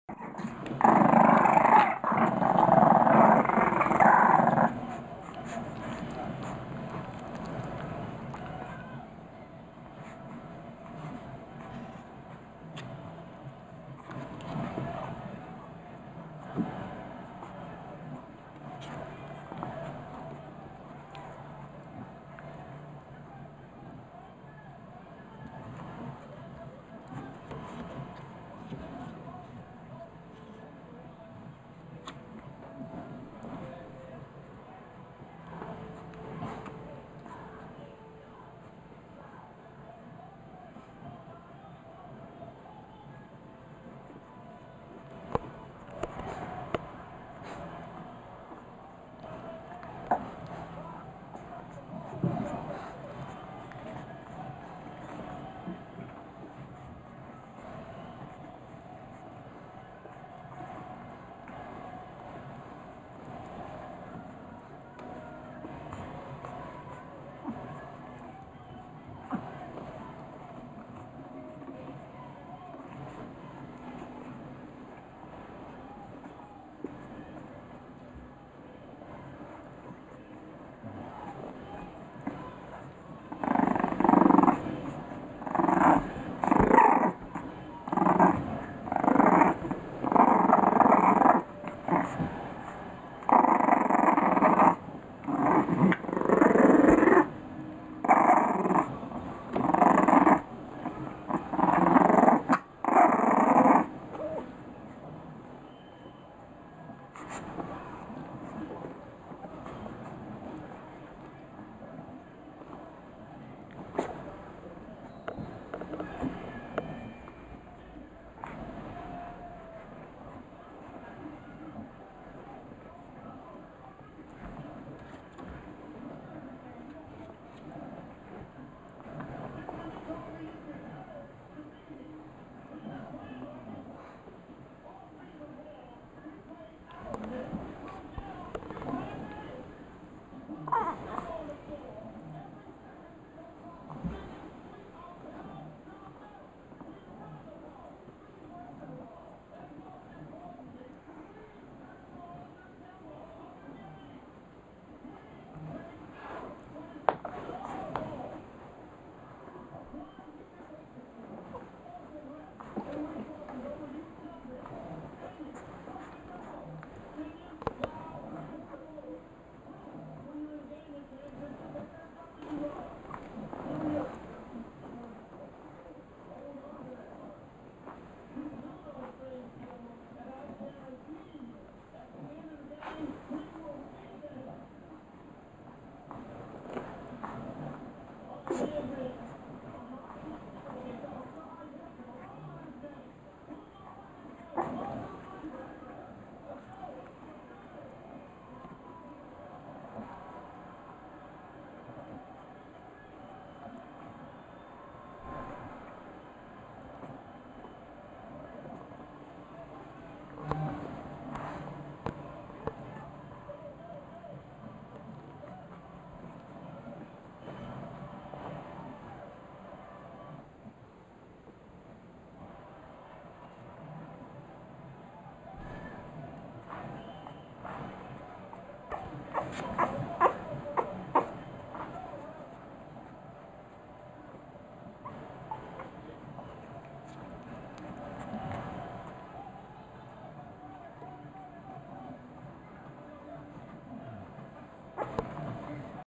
Three week old Bulldog puppies
English Bulldog puppies at three weeks old playing